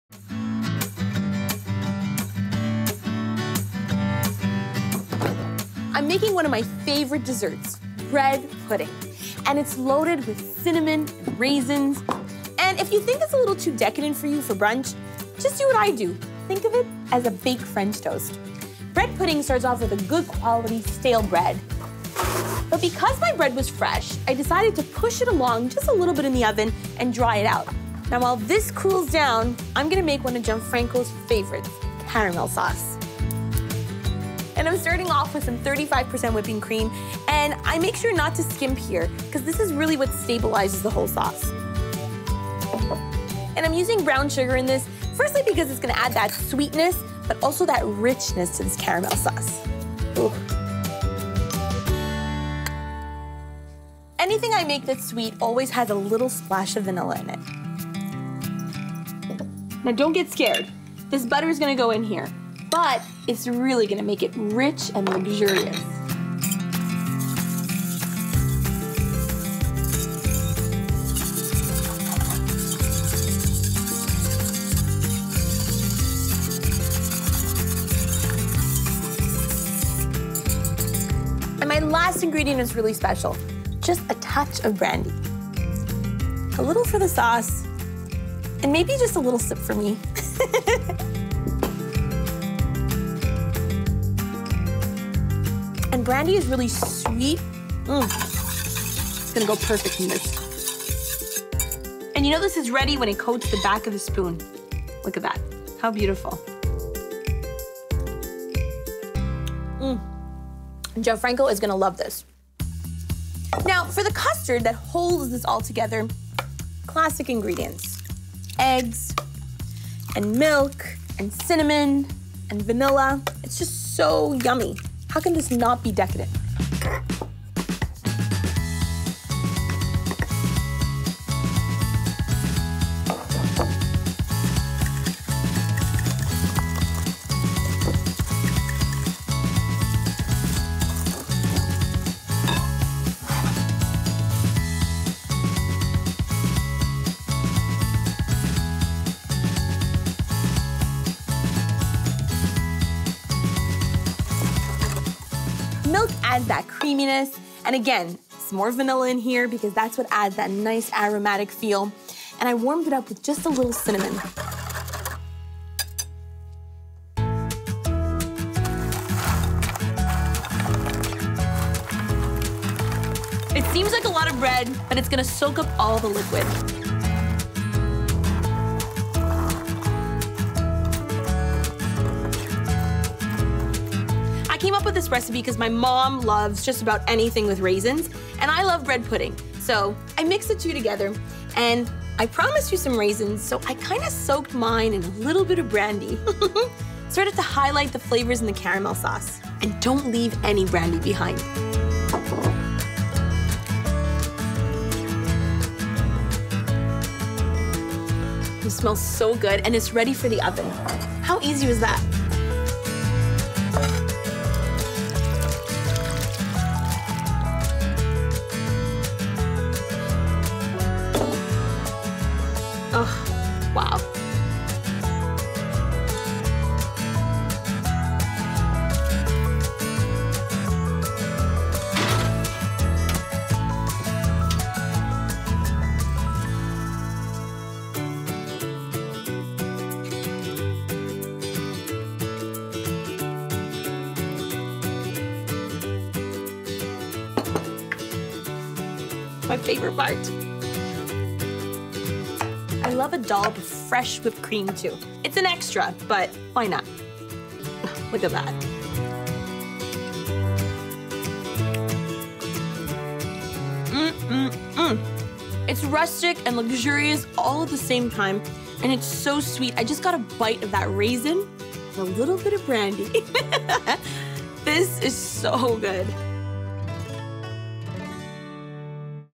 Cinnamon Bread Pudding | Let's Brunch
Fall in love with this cinnamon bread pudding recipe! Drenched in custard with bursts of raisins and brandy, this dish is forkfuls of heavenly bliss.